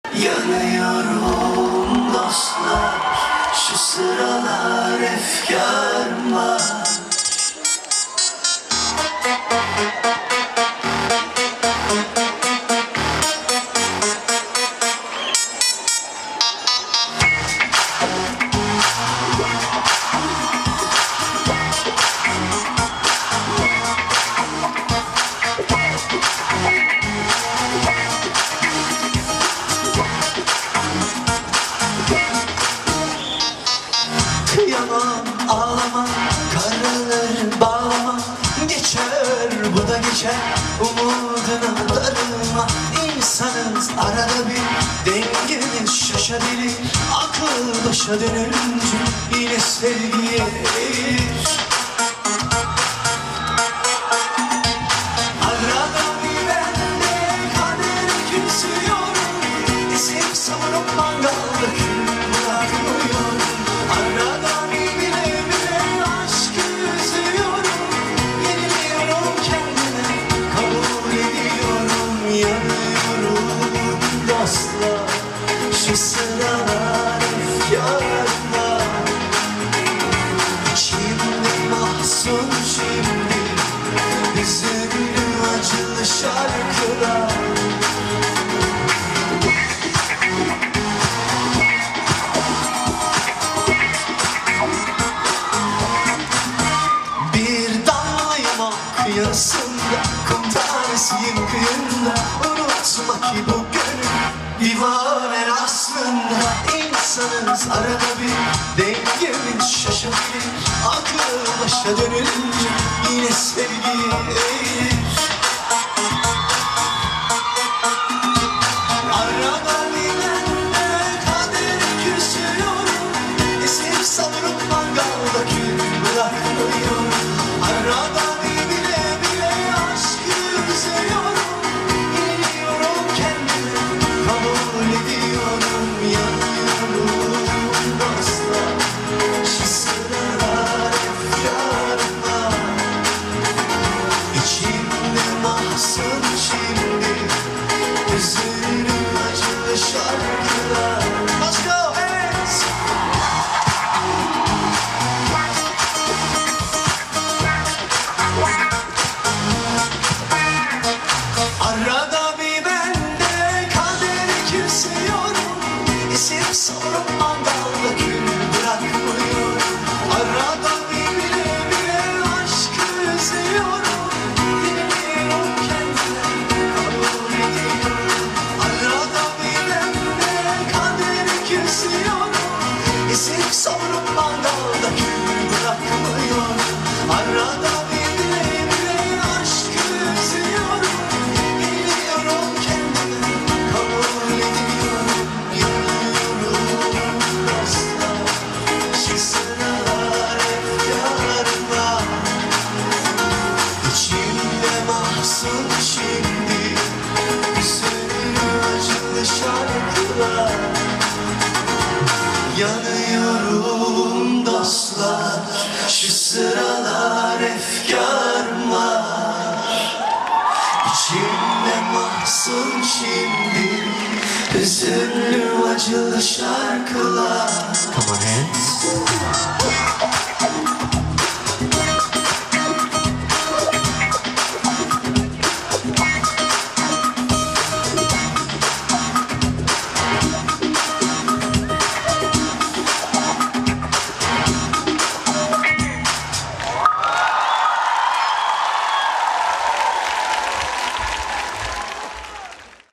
Таркан - концерт в Москве - Tarkan
Видеофрагмент концертного выступления Таркана (Tarkan)  в Крокус Сити Холл 21 ноября 2009 года